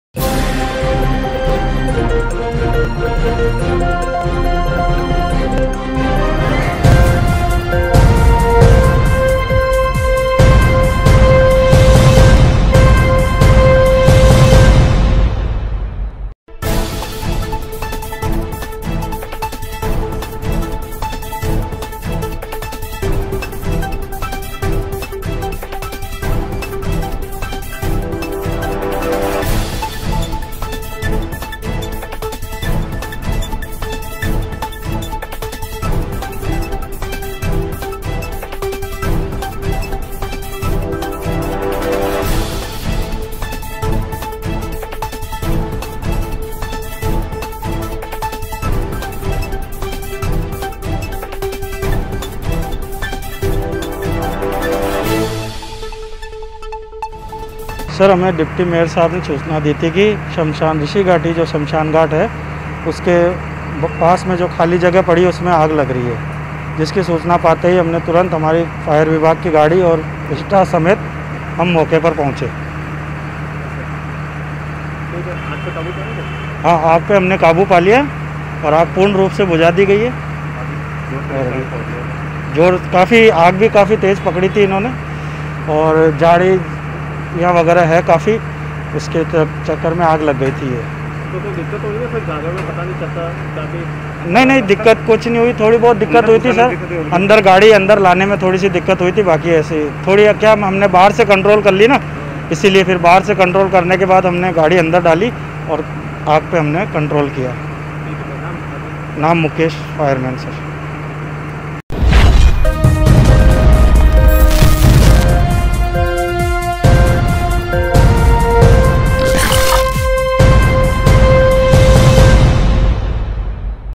ऋषि घाटी स्तिथ समशान घाट के नजदिक लगी आग,आग ने धारण किया विकराल रूप,अग्निशम को दी गयी सूचना ,मौके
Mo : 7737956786